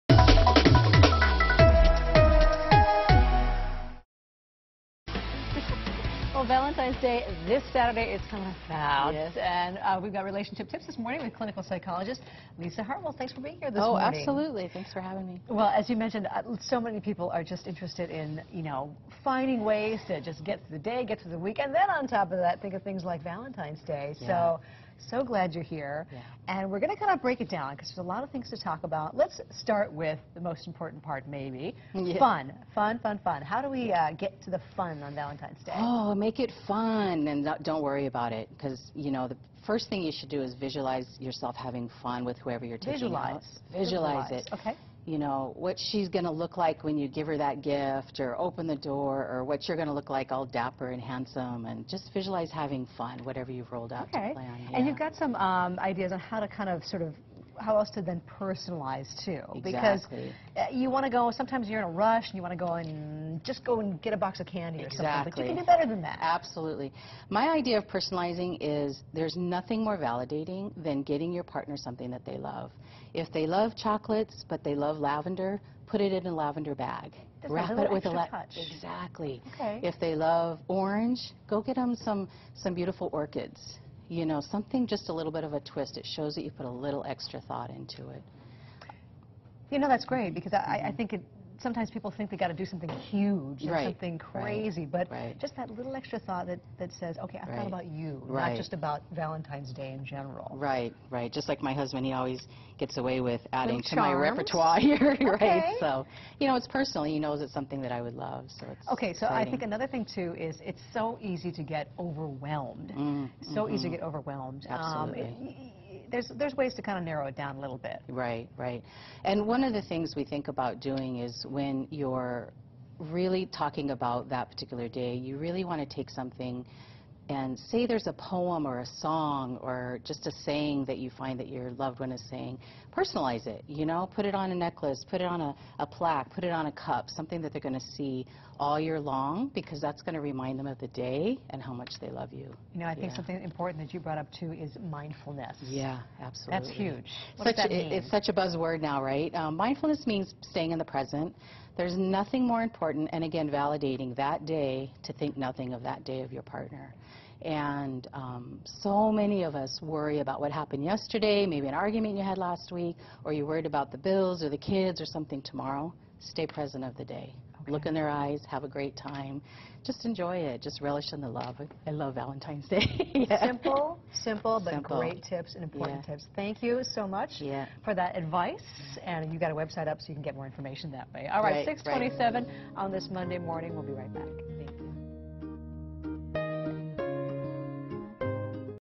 Make this Valentine's Day special for your certain someone
Personalize this years Valentine's Day for your special someone with a few helpful tips.